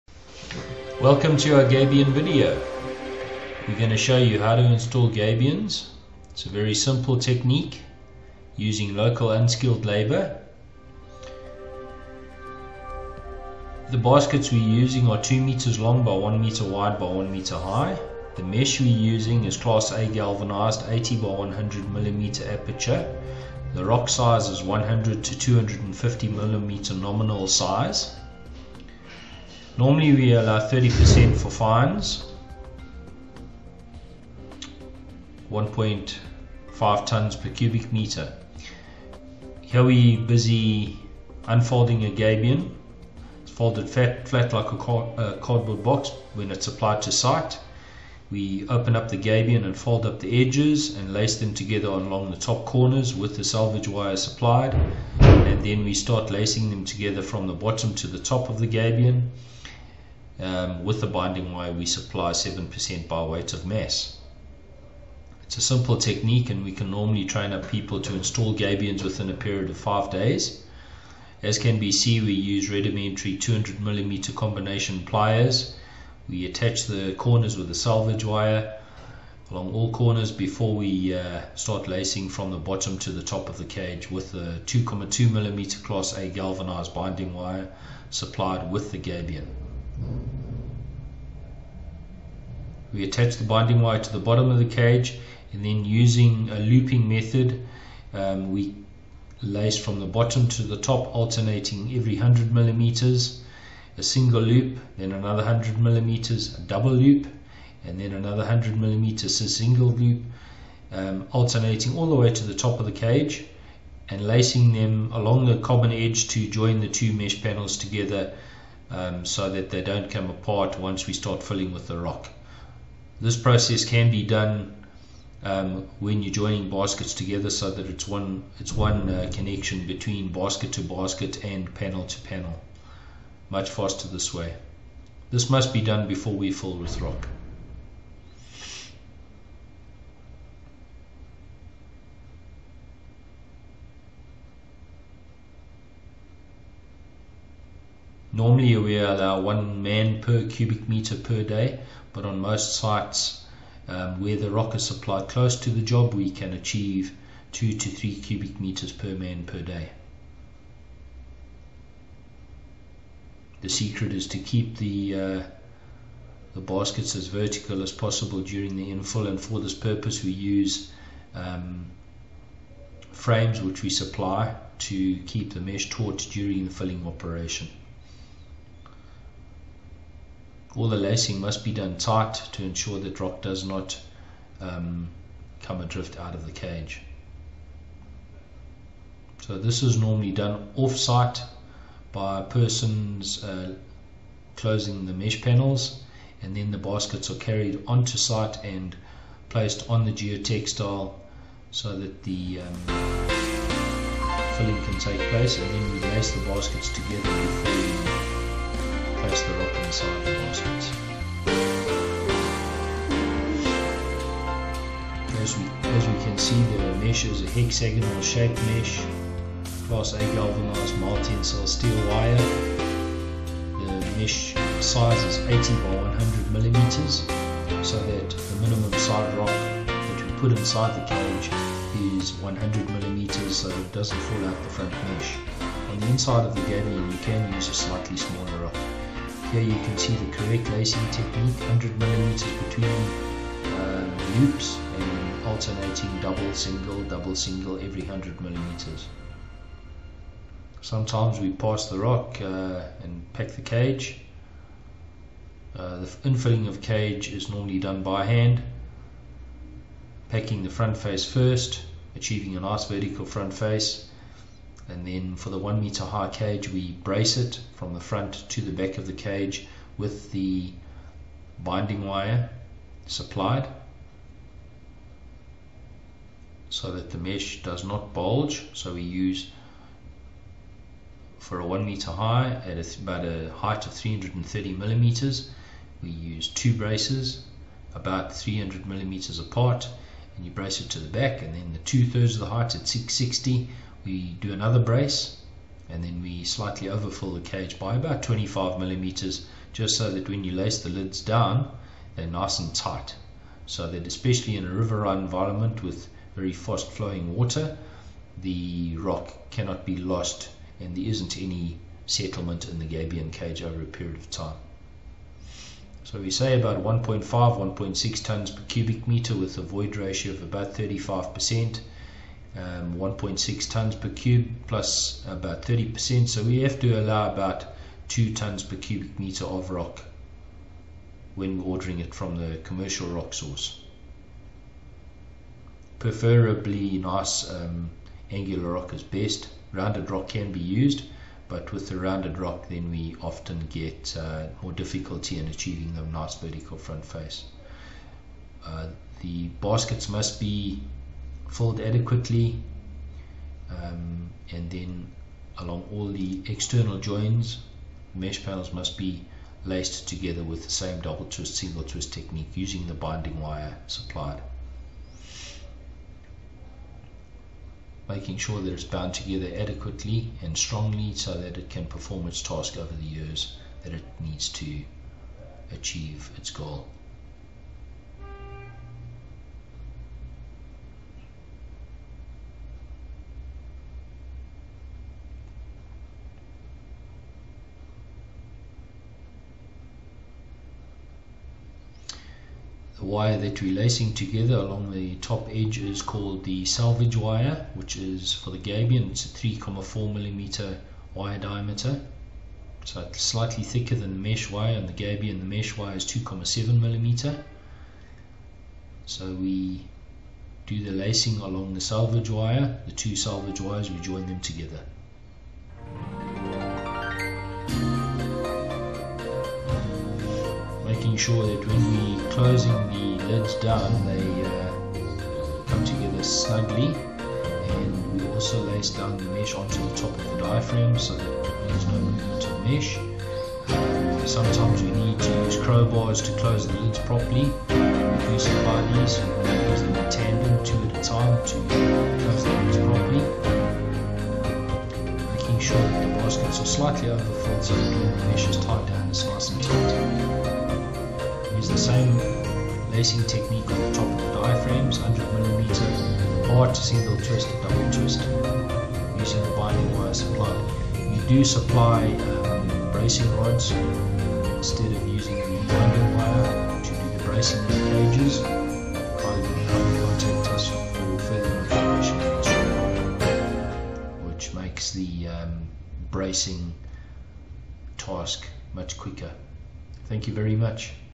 Gabion Installation Video
How to install a gabion wall structure, it's simple, economical and environmentally friendly, and we offer toolsets and on site training as required, kindly contact us for further information based in Johannesburg, South Africa.